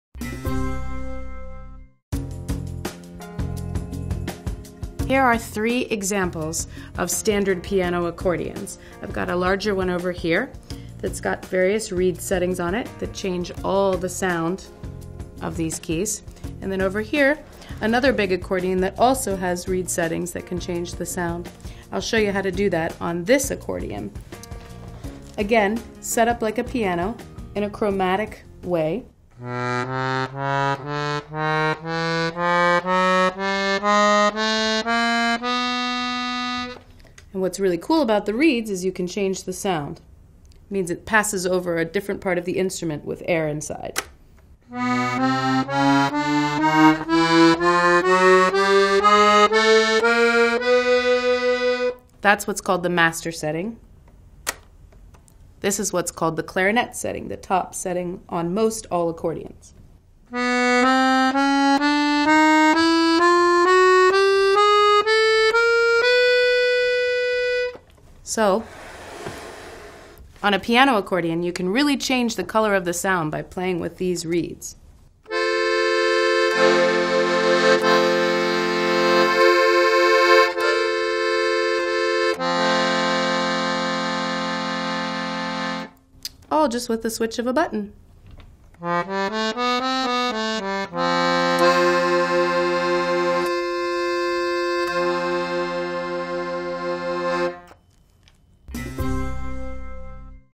How to Play a Piano Accordion | Accordion Lessons
Here are three examples of standard piano accordions.

I've got a larger one over here that's got various reed settings on it that change all the sound of these keys. And then over here another big accordion that also has reed settings that can change the sound.

I'll show you how to do that on this accordion. Again, set up like a piano in a chromatic way. And what's really cool about the reeds is you can change the sound. It means it passes over a different part of the instrument with air inside.

That's what's called the master setting. This is what's called the clarinet setting. The top setting on most all accordions.

So, on a piano accordion you can really change the color of the sound by playing with these reeds. All just with a switch of a button.